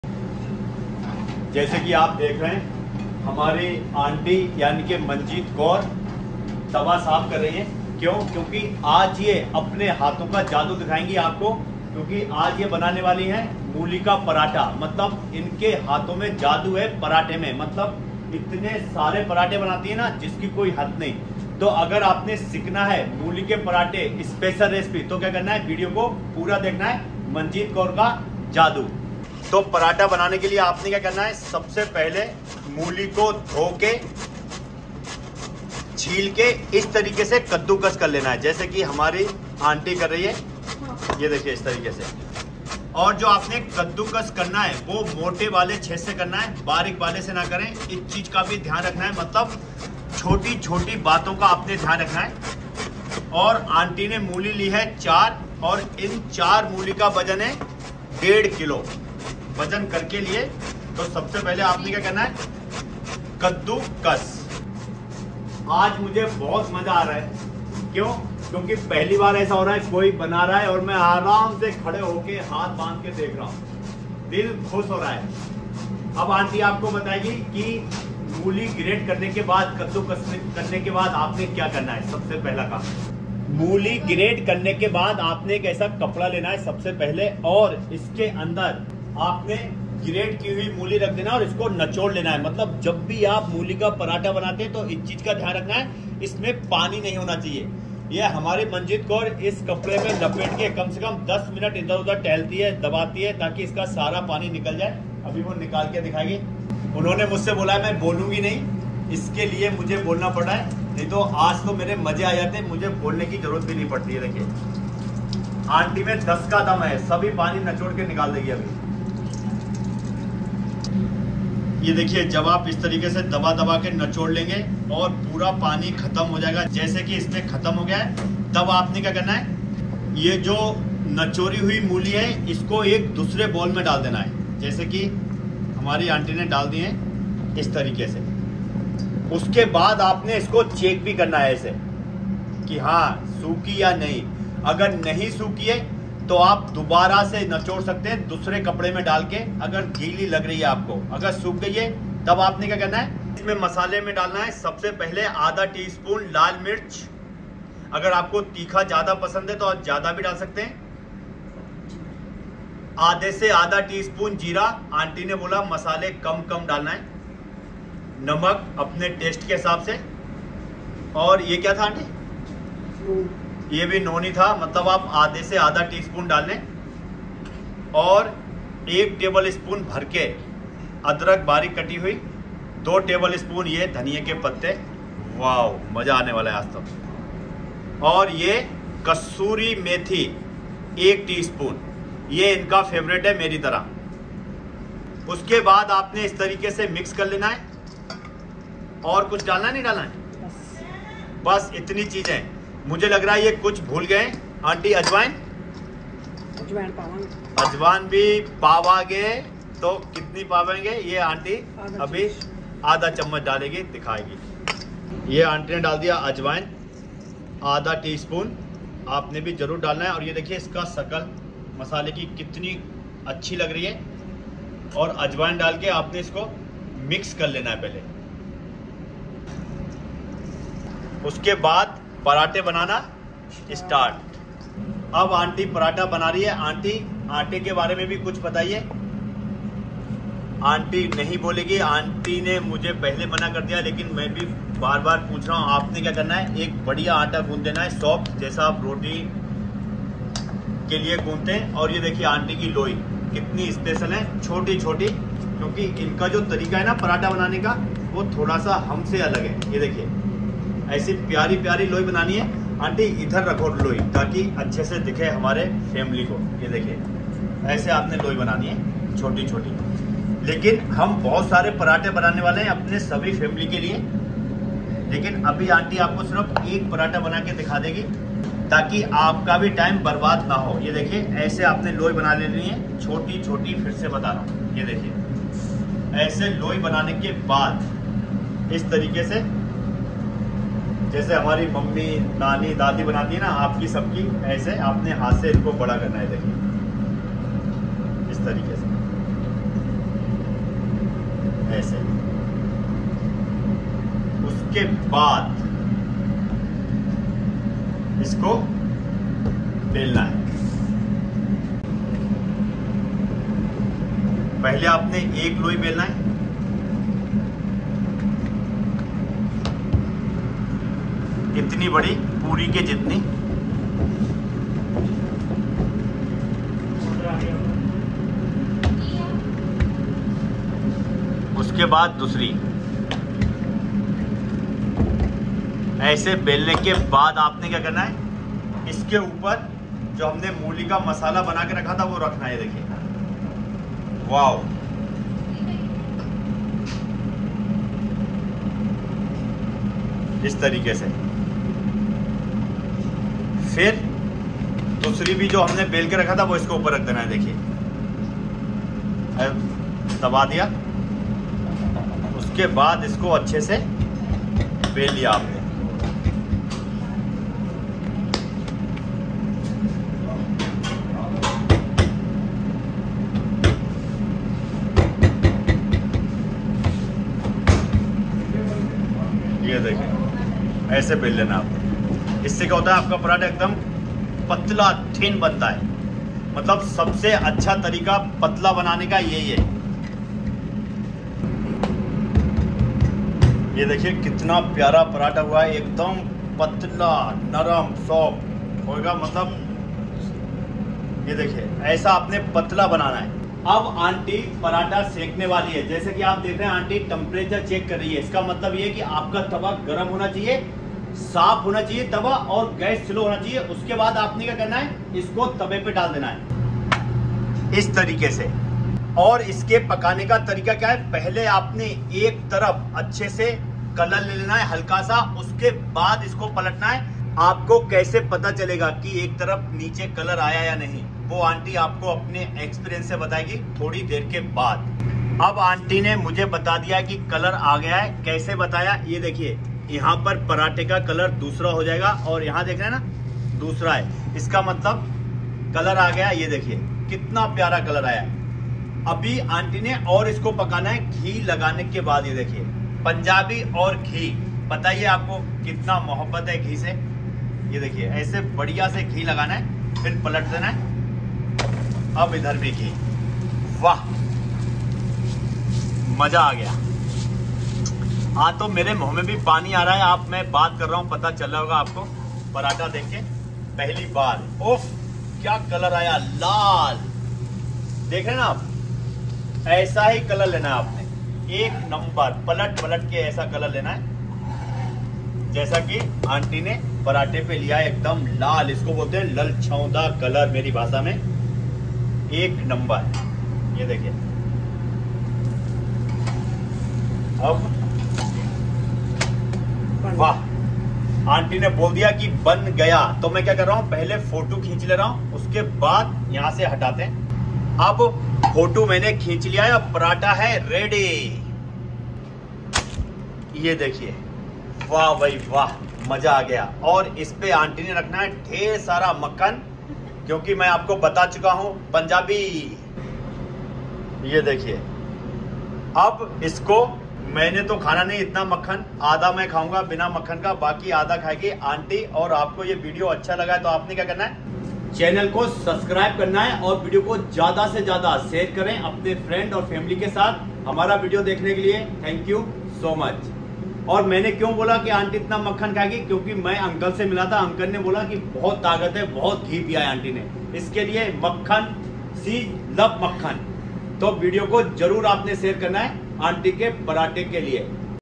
Mooli Ka Paratha | Aunty Ke Hatho Ka Special Mooli Paratha Recipe | Stuffed Paratha Recipe
INGREDIENTS:
Radish= 1500 gm
Clarified Butter